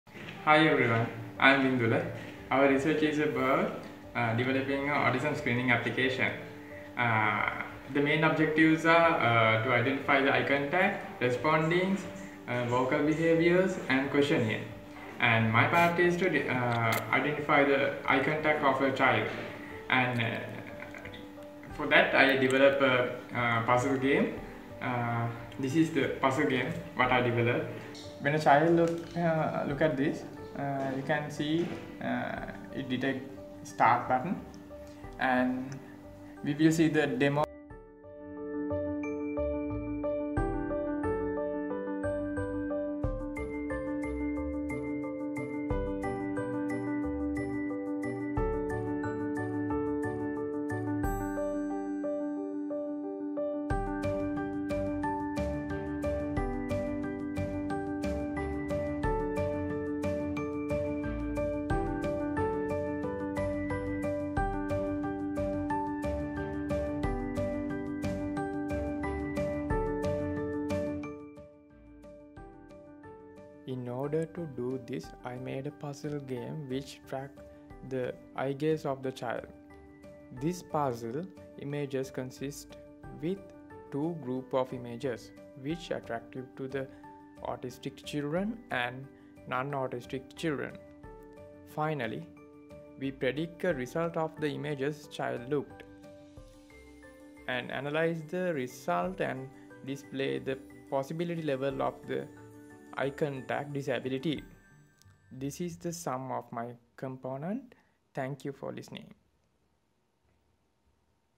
Dreampad | Autism Screening Application | Eye Contact
Eye Contact Identification Component of our Autism Screening Application research. This is my 4th-year research component demo video of 'Dreampad' android application.